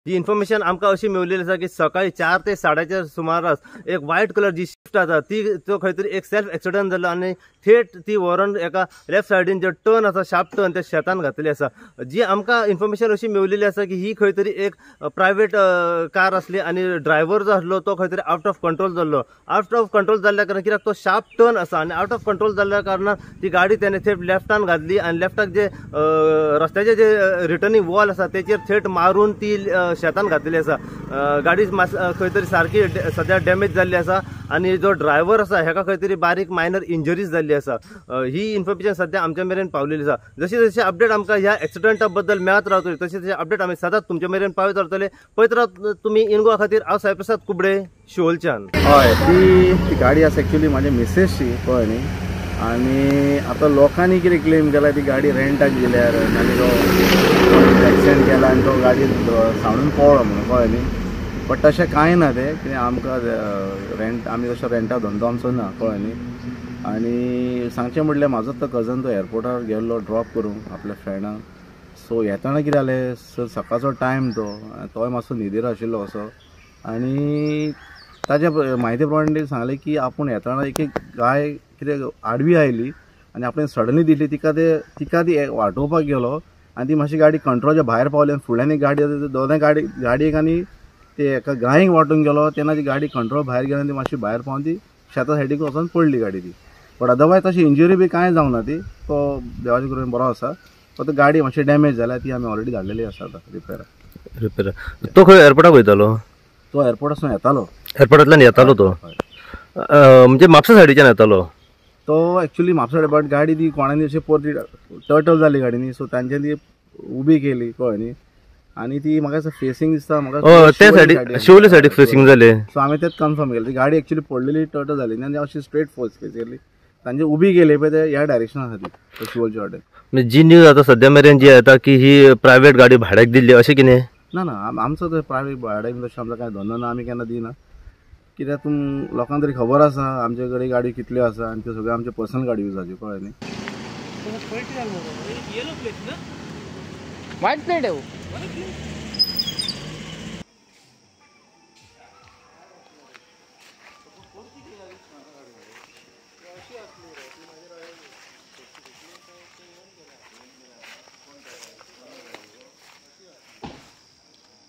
Car driver loses control and veers off road into fields.
Car driver loses control and veers off road into fields. Locals claim pvt vehicle was given on rent; Owner denies